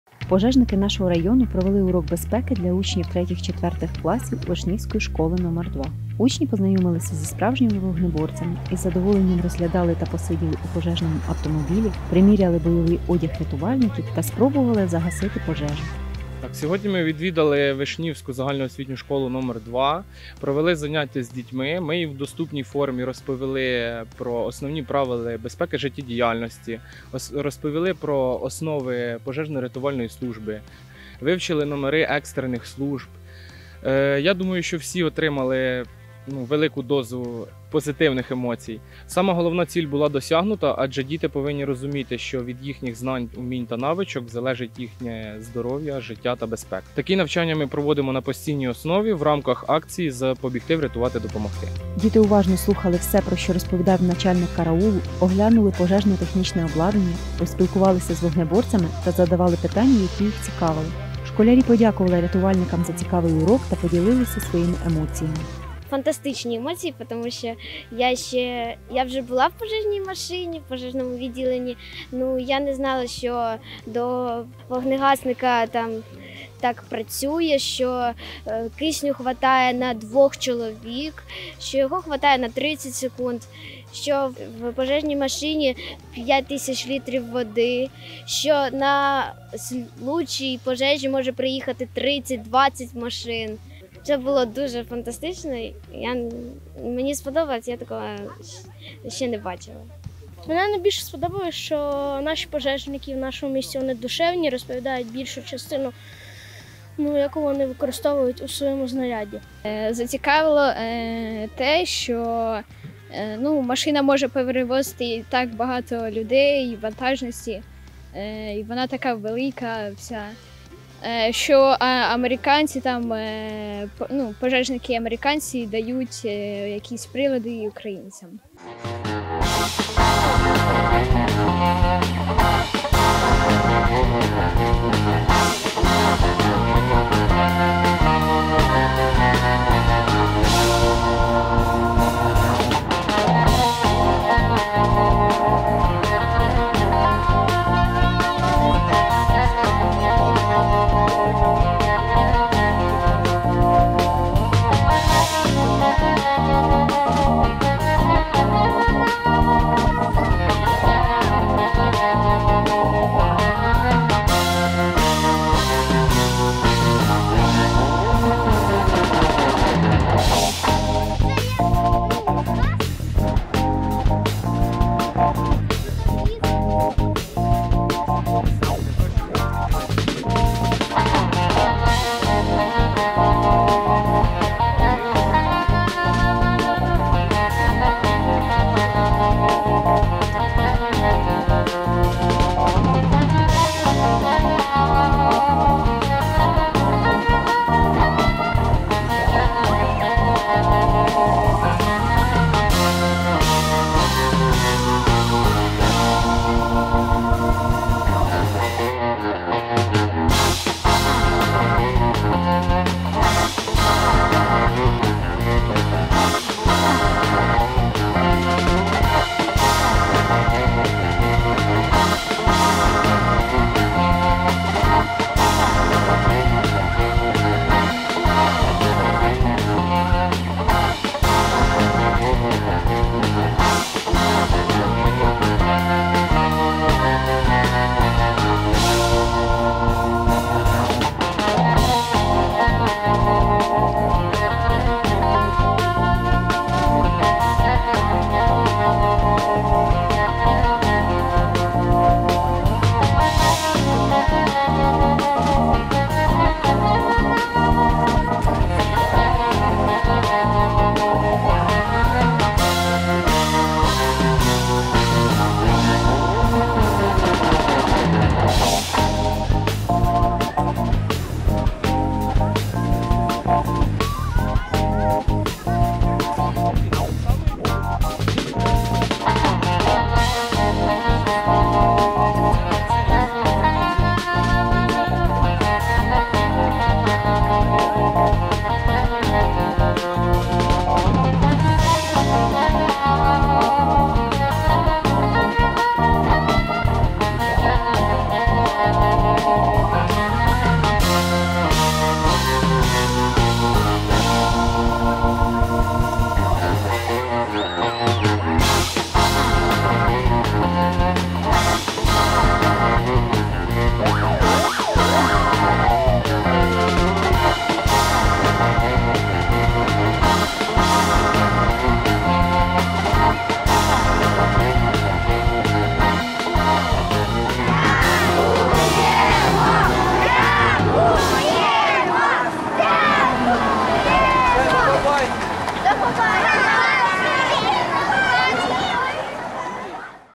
Пожежники у Вишнівській школі № 2
Пожежники нашого району провели урок безпеки для учнів 3-4 класів Вишнівської школи № 2.
Учні познайомилися з справжніми вогнеборцями, приміряли бойовий одяг рятувальників. із задоволенням посиділи у пожежному автомобілі та спробували себе у ролі пожежника.
Школярі уважно слухали про все, що розповідав начальник караулу, оглянули пожежно-технічне обладнання, поспілкувалися з вогнеборцями та задали питання, які їх цікавили.
Діти подякували рятувальникам за цікавий урок та поділилися своїми емоціями.